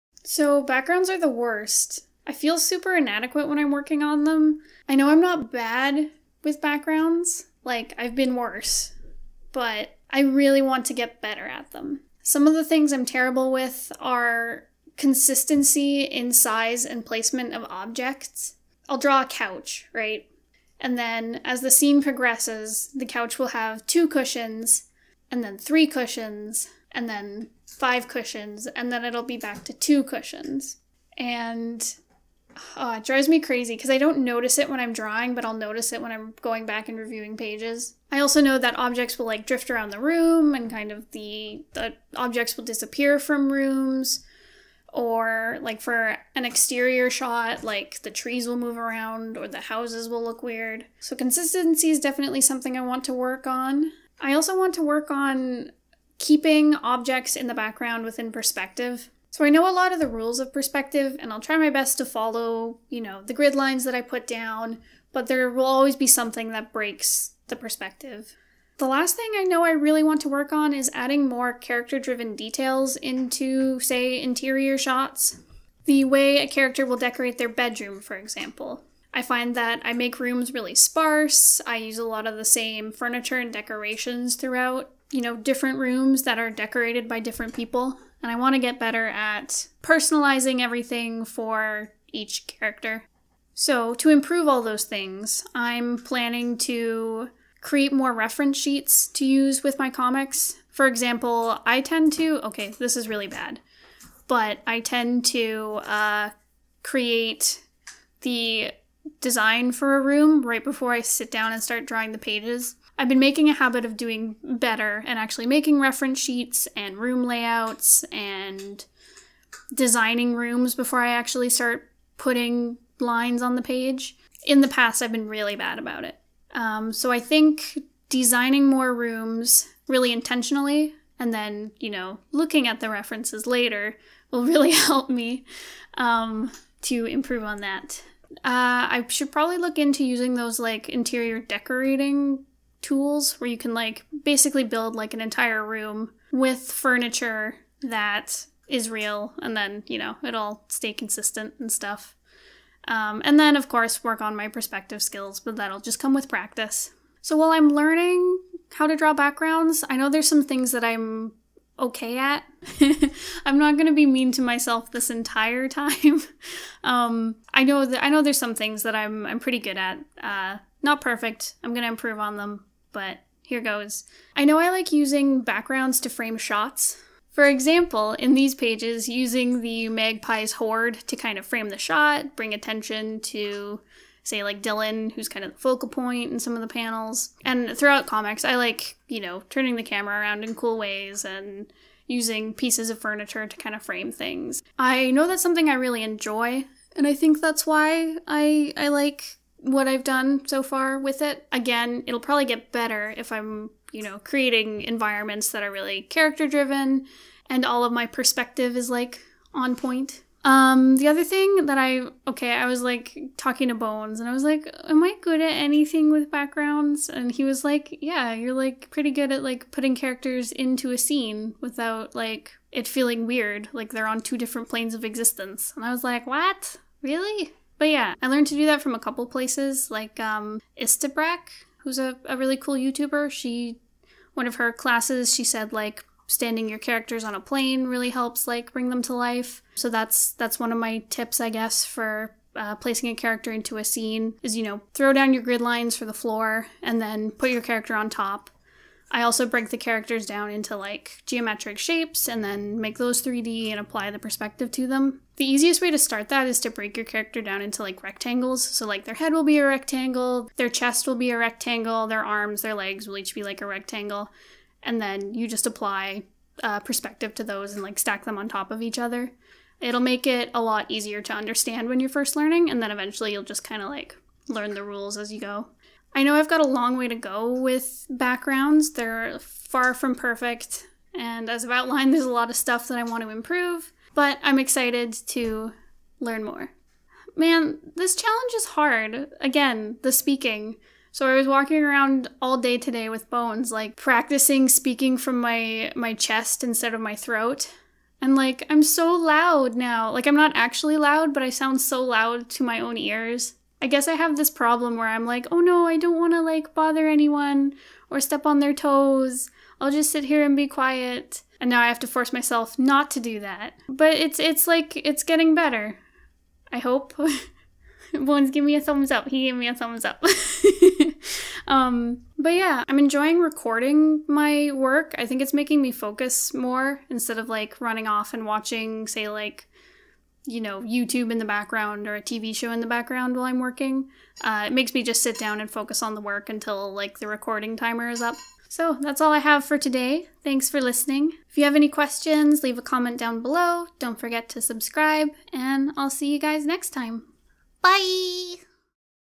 Struggling with Backgrounds!!! - 100 Days of Making Comics - DAY 3
Please listen to me while I whine about backgrounds.